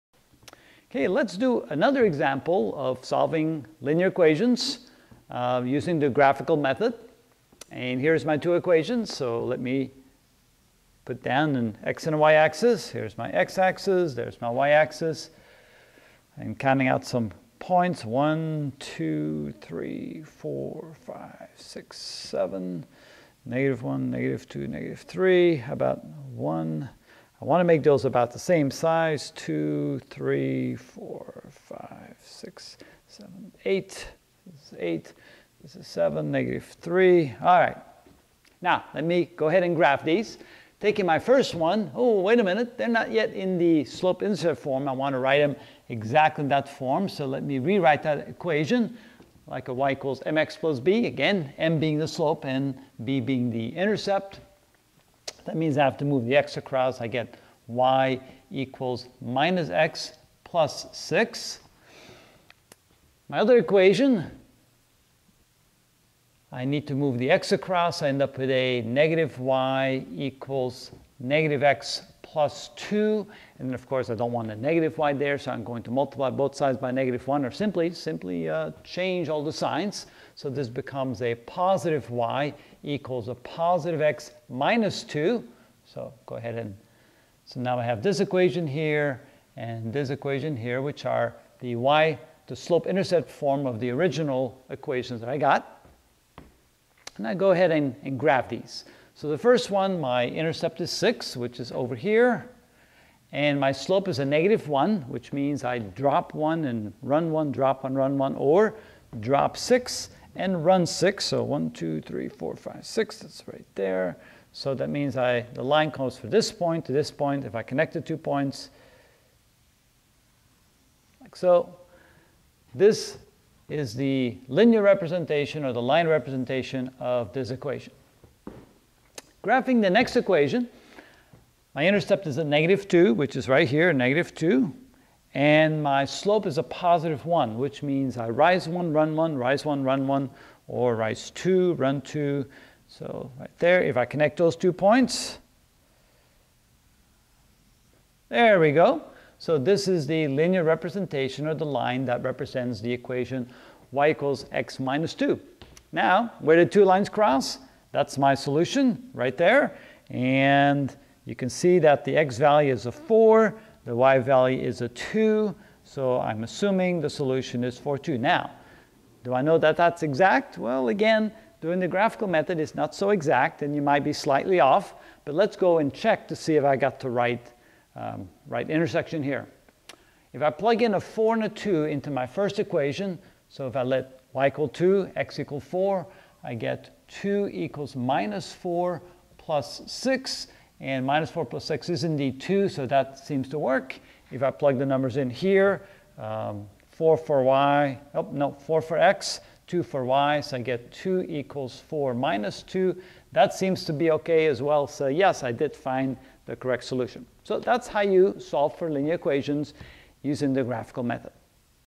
Algebra - Solving Linear Equations by using the Graphing Method 2/2
In this lecture series I'll show you how to solve for multiple variables simultaneously using the technique called: the Graphing Method